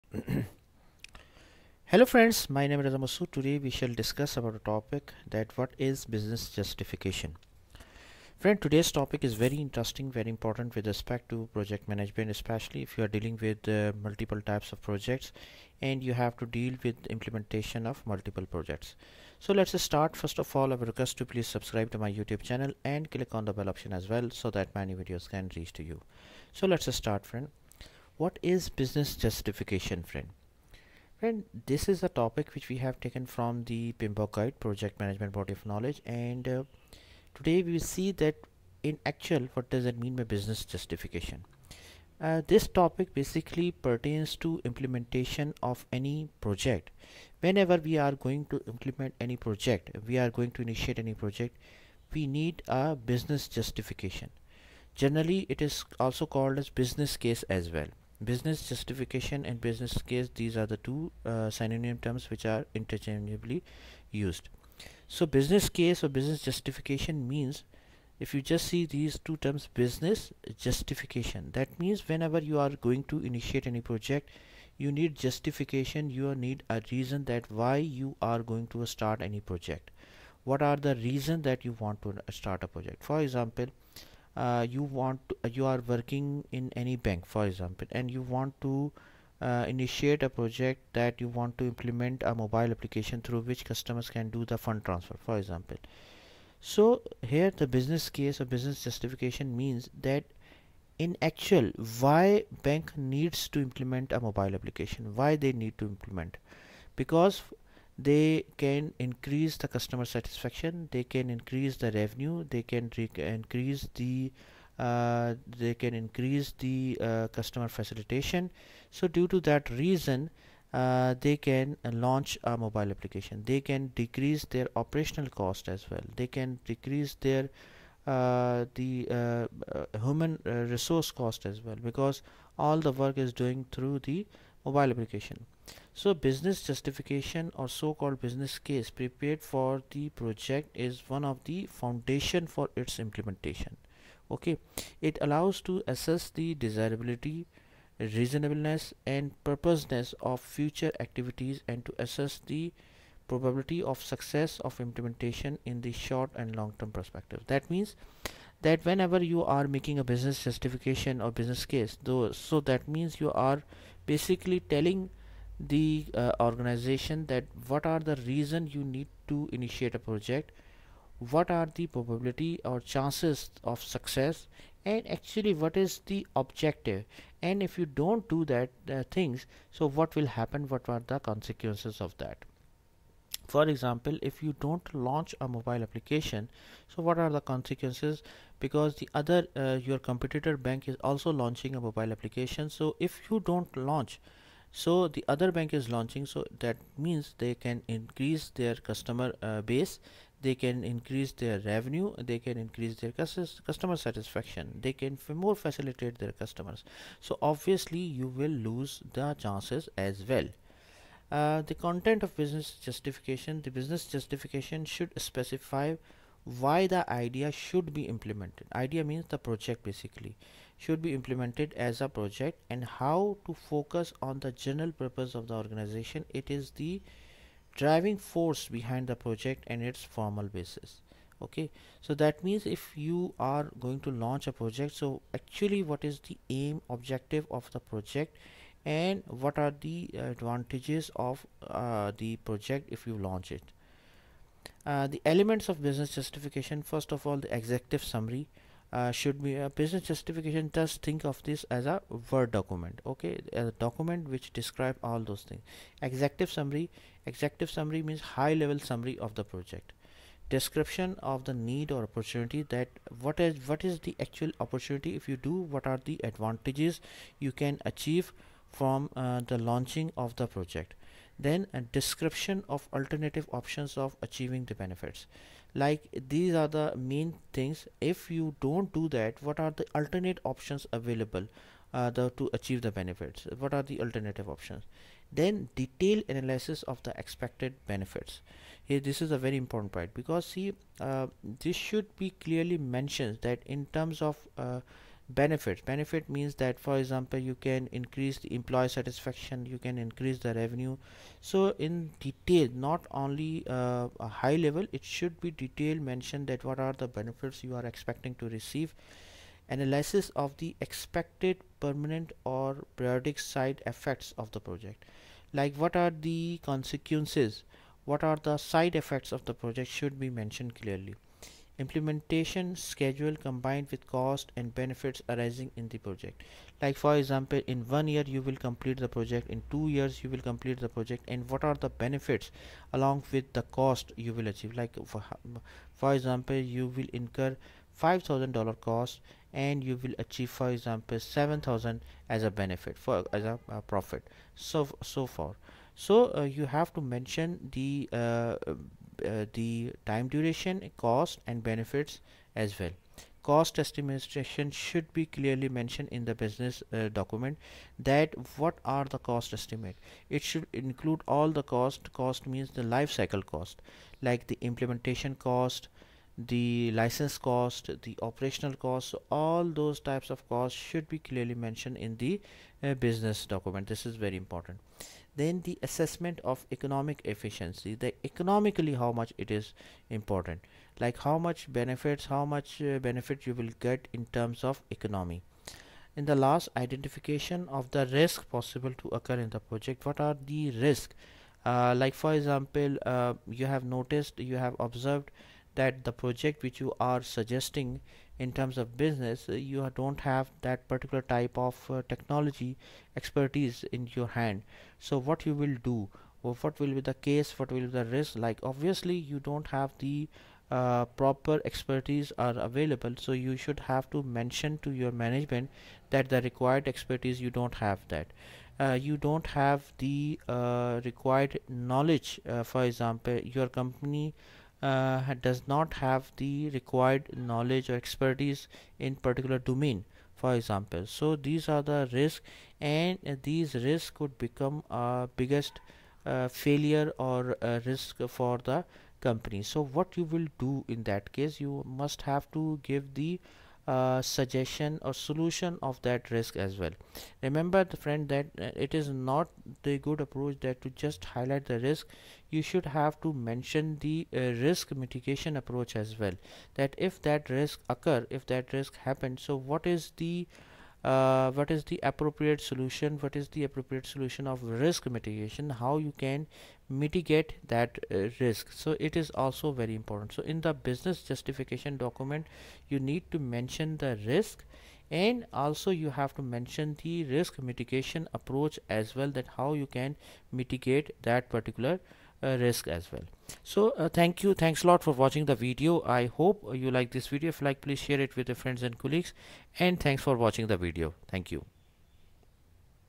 Business Justification
Business justification or so called business case prepared for the project is one of the foundations for its implementation. It allows to assess the desirability, reasonableness and purposefulness of future activities and to assess the probability of success of implementation in the short and long-term perspective. The business case of the project is also a document allowing the entities and institutions supervising the project (stakeholders) to take and justify decisions having an impact on the course of the project. This is one of the essential parts of the basic document initiating the project: Project Charter. To a minimum extent, business justification defines the expected results of the project and gives direction to the project team